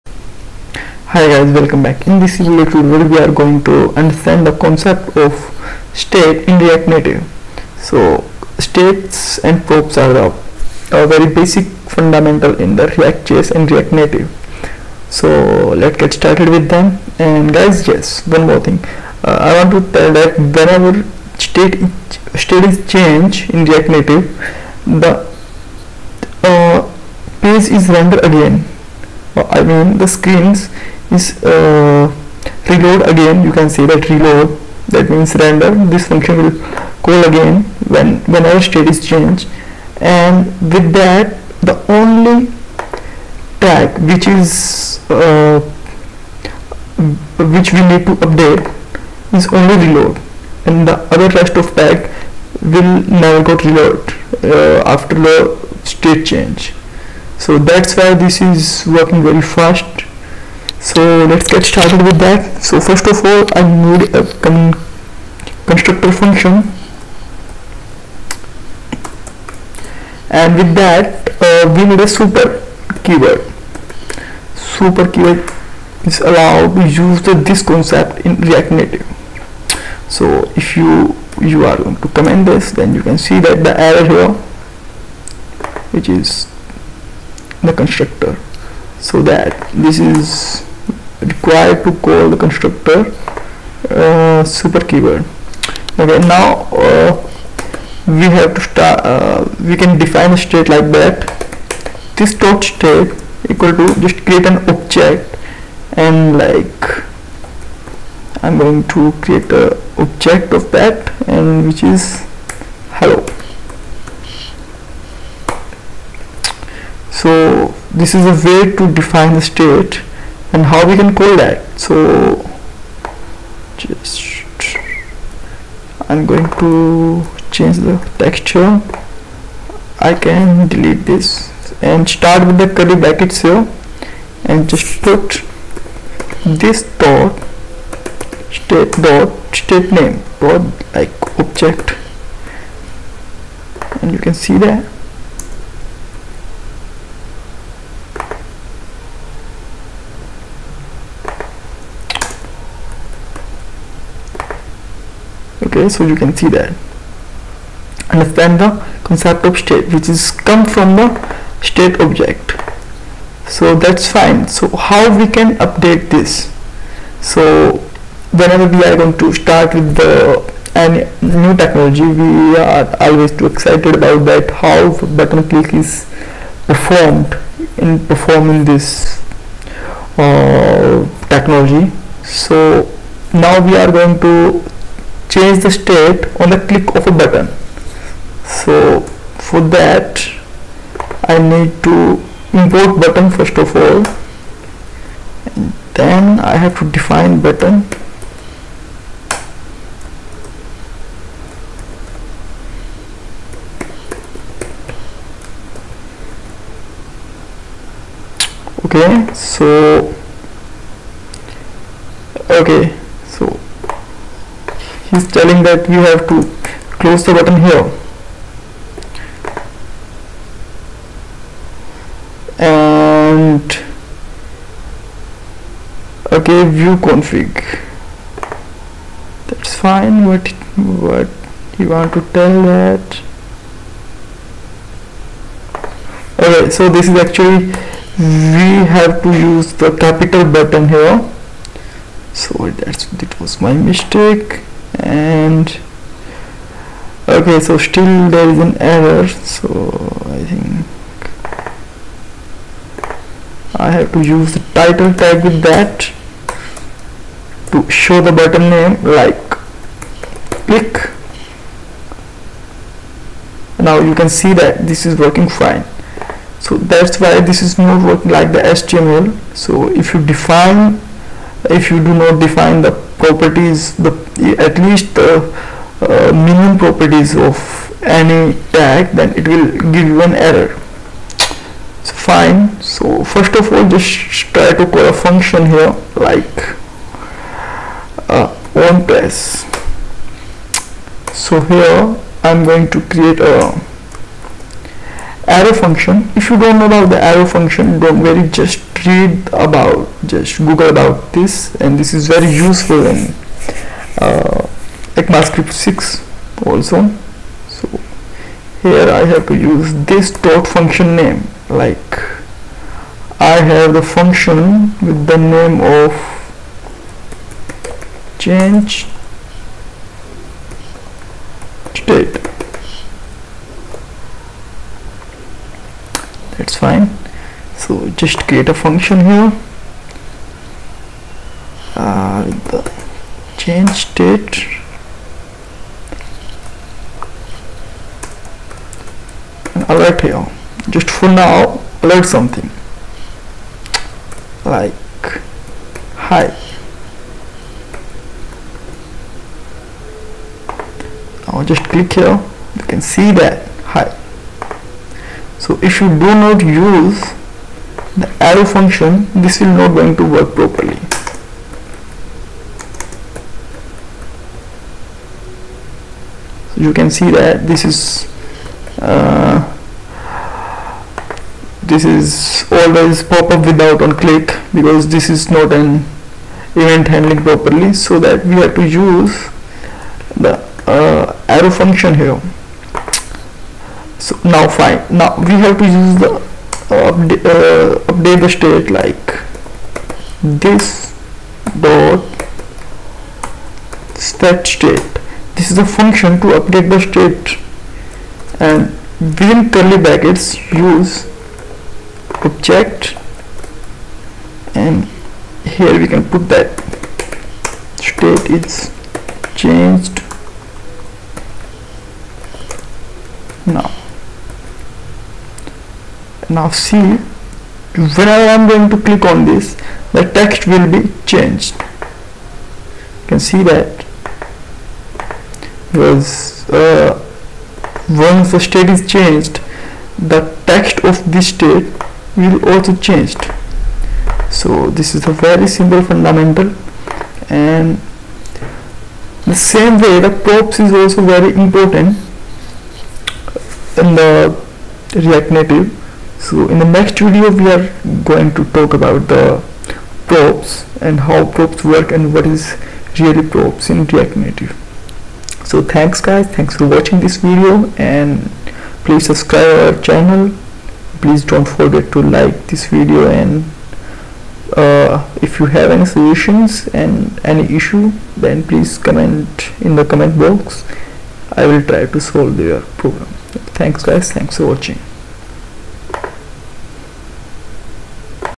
React-native tutorial # 3 understand the concept of state
state is important fundamental in react-native and this tutorial we are going to understand this concept and we will  update this with a function.
like use on facebook here you can get video with code